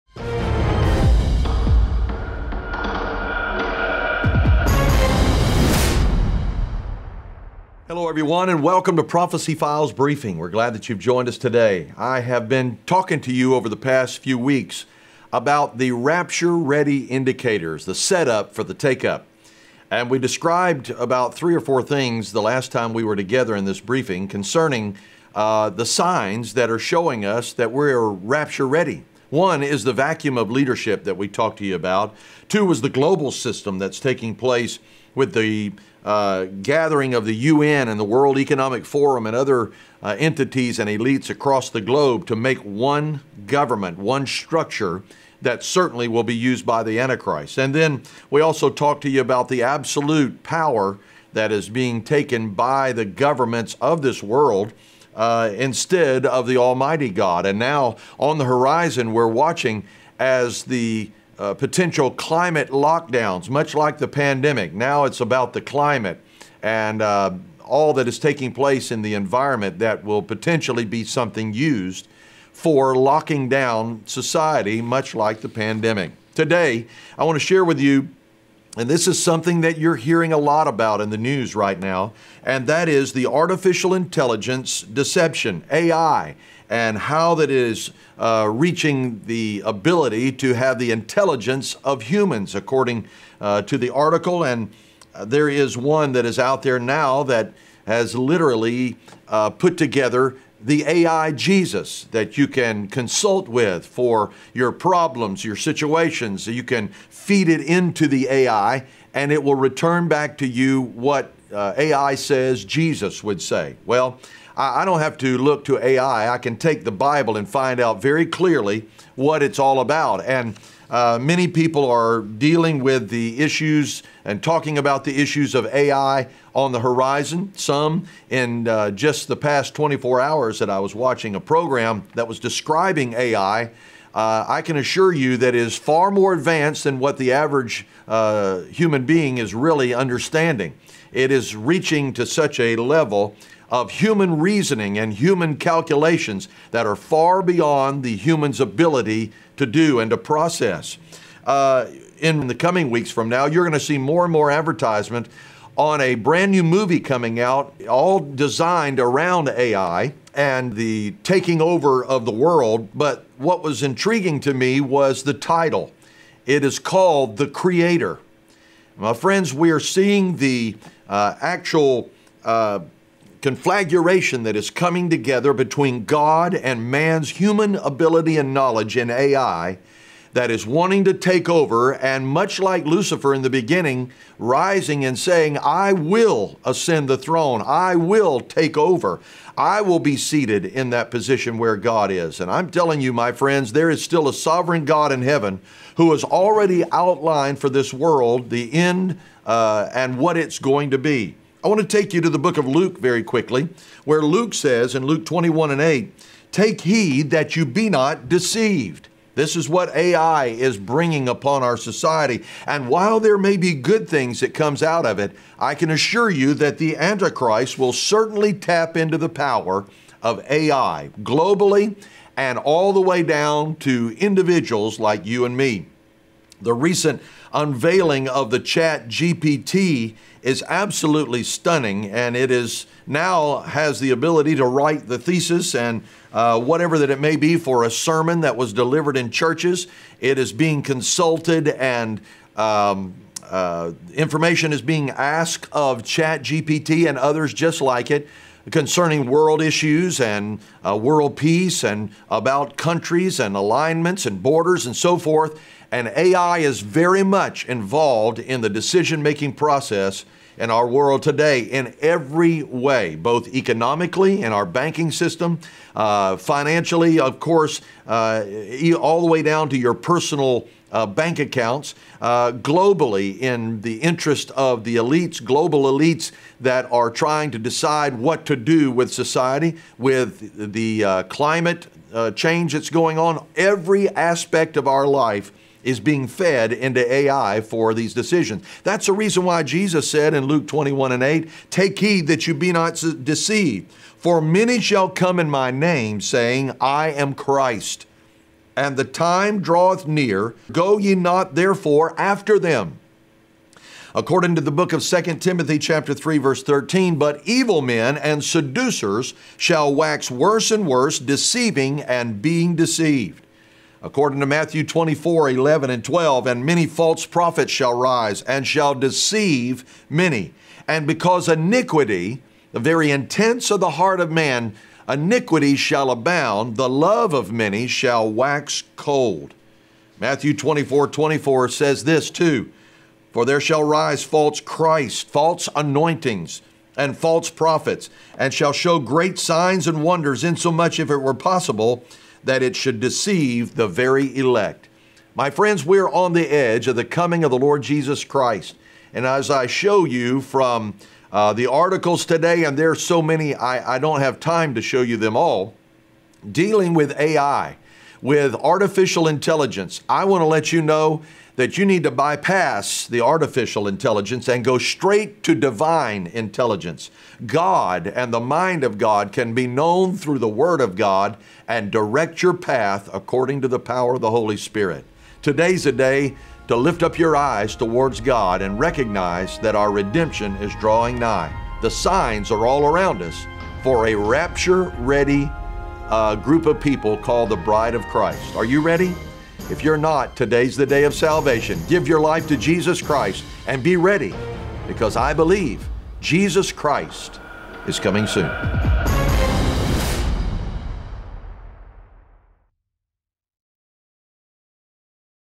Prophecy Files Briefing
Join Pastor Joey Rogers for this week's briefing as he discusses the current state of A.I. and how it is shaping our society.